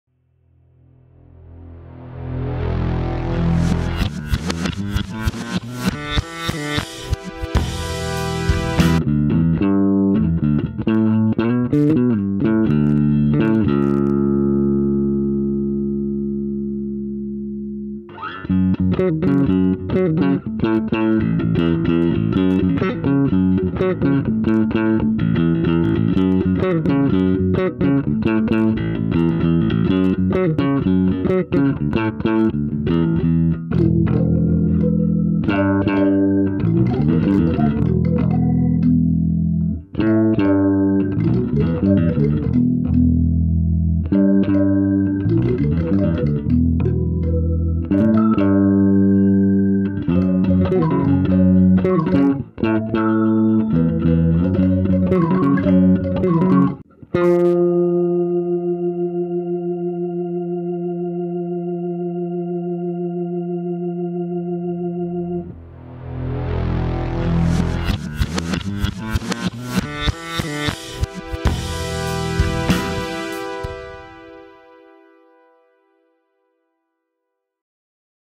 Bass Guitar + MIDI Using Helix, Sonuus i2m and Mainstage
I used my Helix LT to route my bass signal to the effects send 1/4" and then into my Sonuus i2m pitch to MIDI interface.  That connects to my Mac via USB and then into MainStage running a Hammond B3 patch.